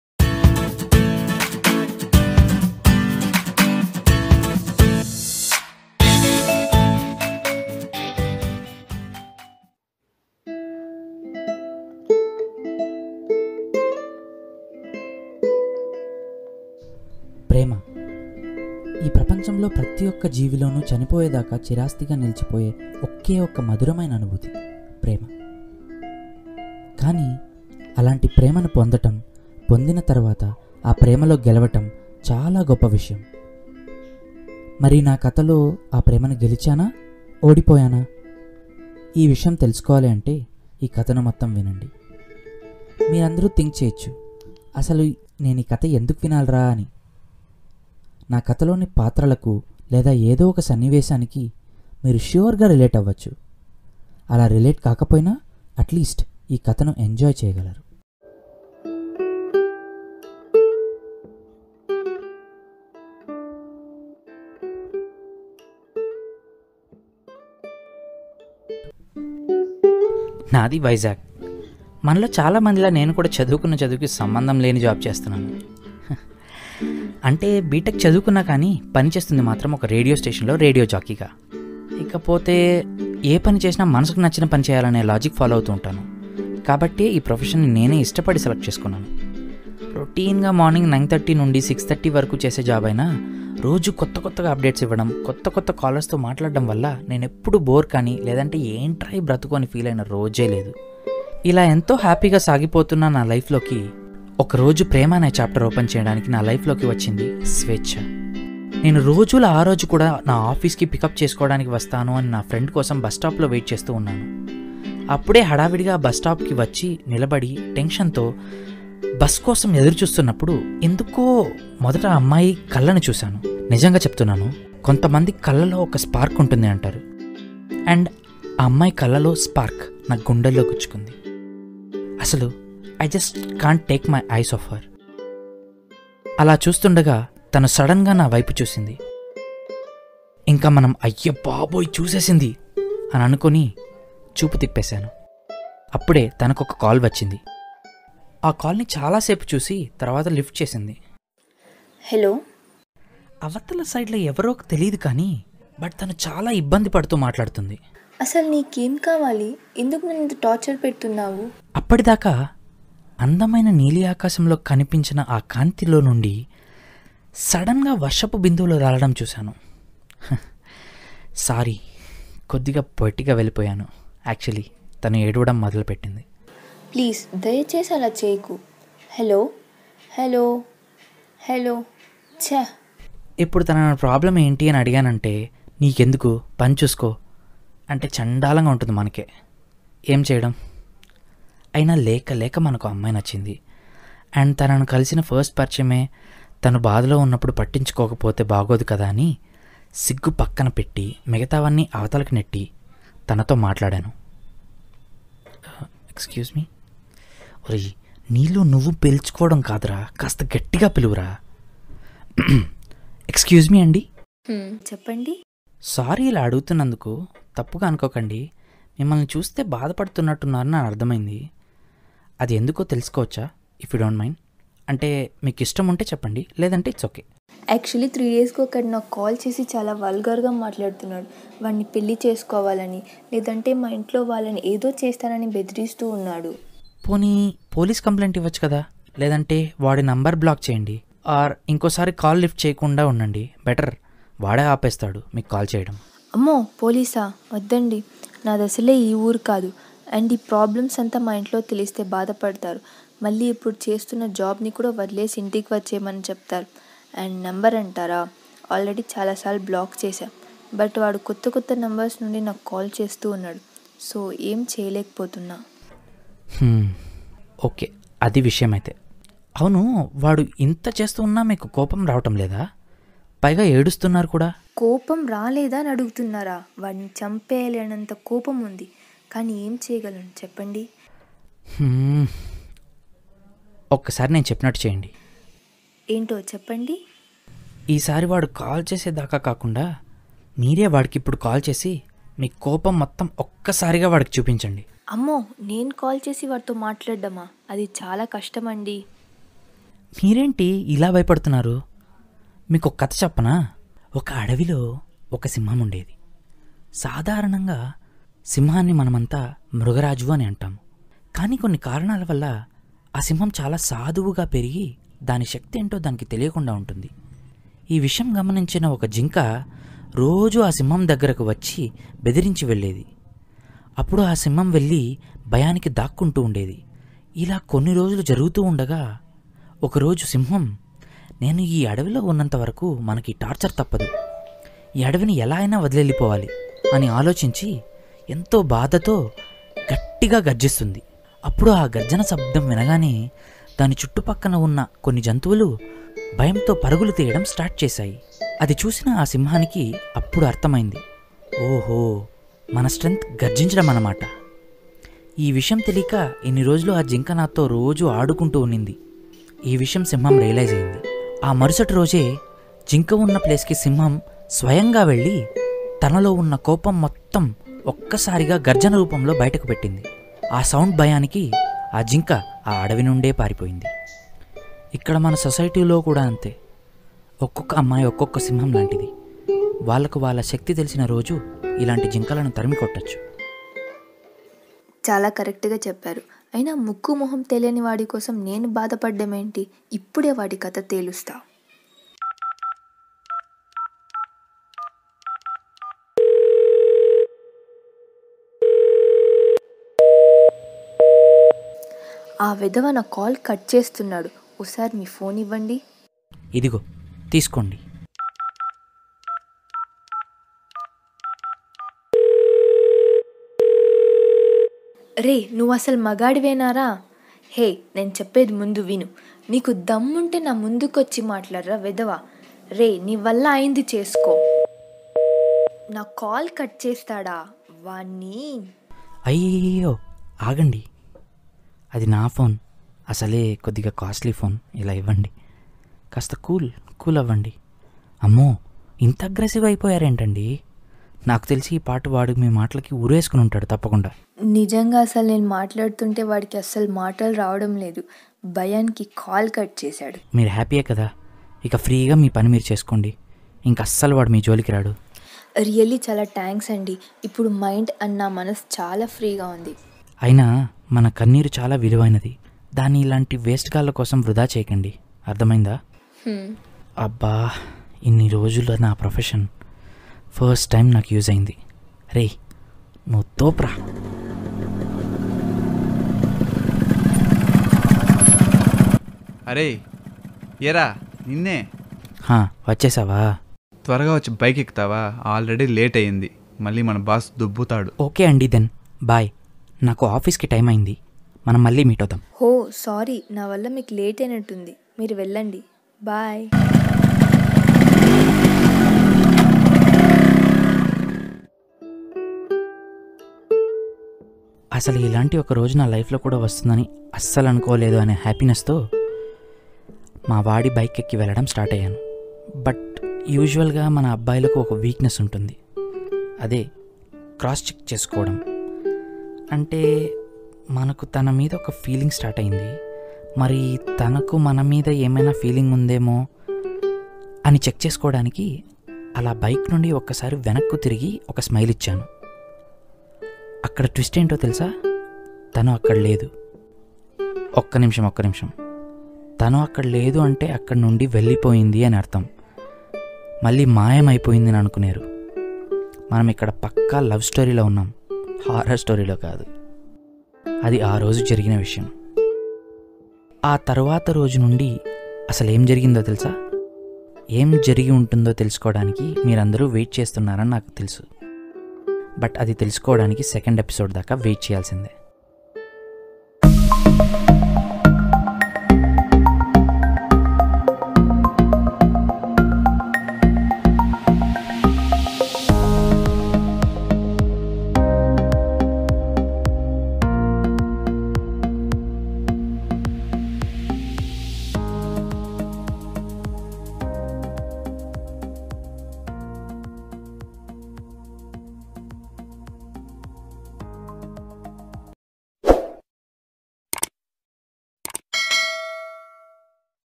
Telugu Love Story | Idhi Naa Prema Katha Episode-1 | ఇది నా ప్రేమకథ
The Telugu Love Story has started from this episode.

Idhi Naa Prema Katha Episode-1

Here we are bringing you the feel good love story in artistic way. Sure you all will enjoy this story.

Male Voice: Nitish
Female Voice: Likitha
Arts: Roshini
Story & Editing: Kranthi